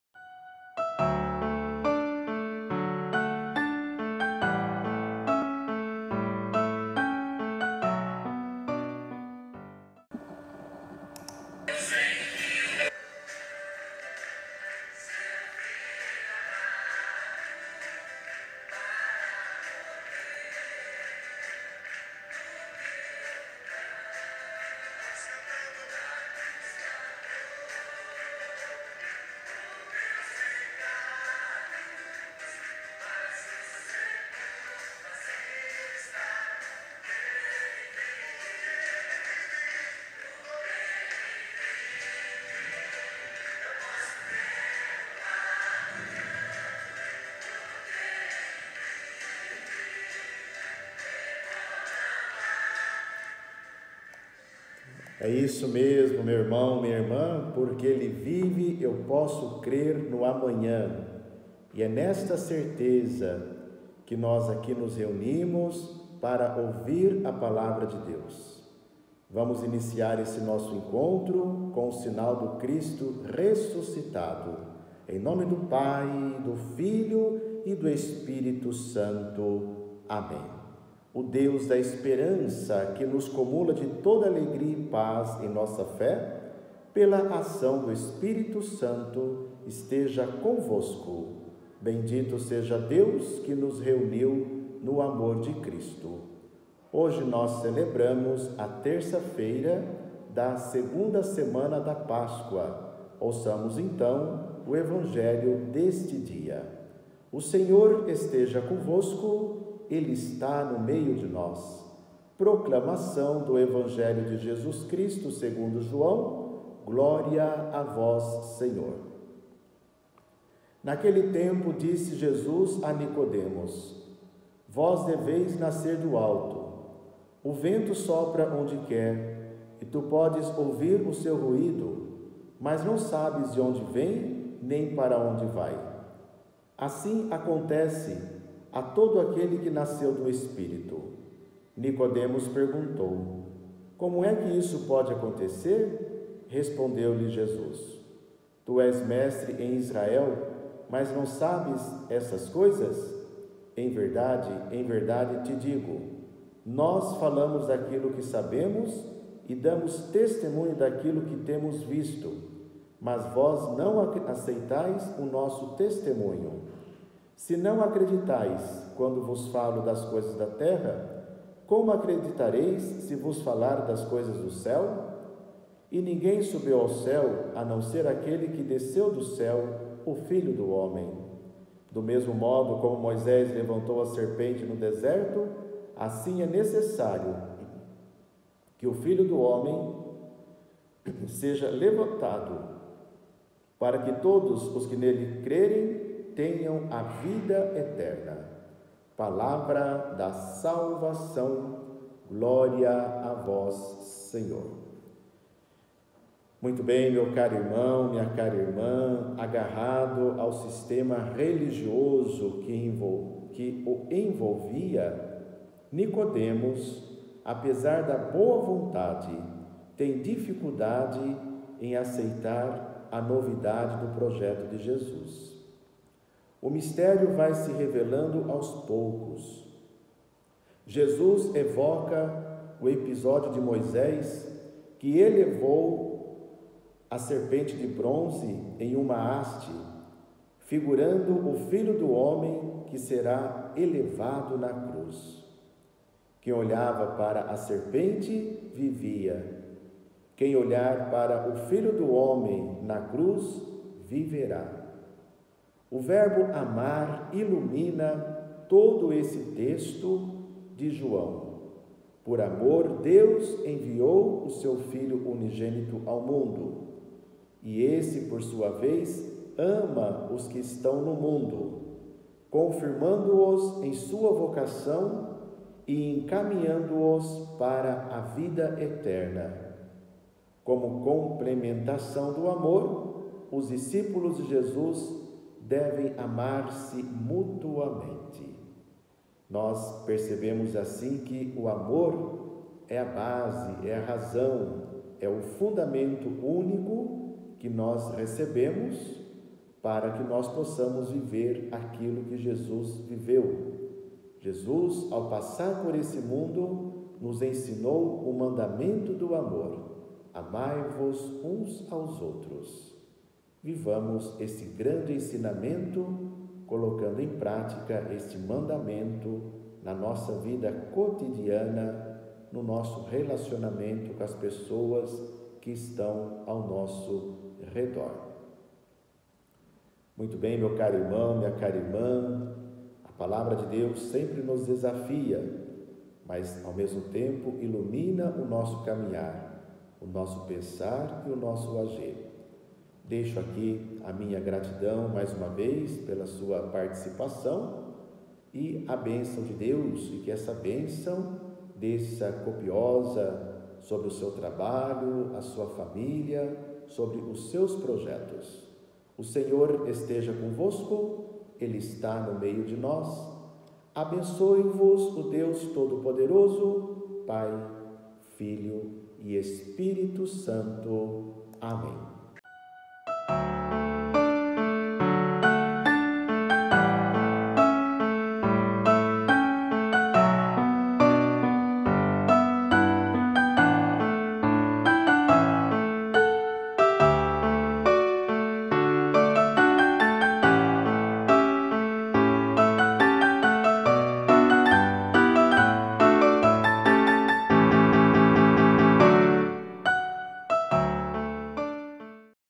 Reflexão do Evangelho (13/04/2021) - Pe. Paulo
Leitura do Evangelho do dia 13/04/2021 + breve homilia - Padre Paulo César.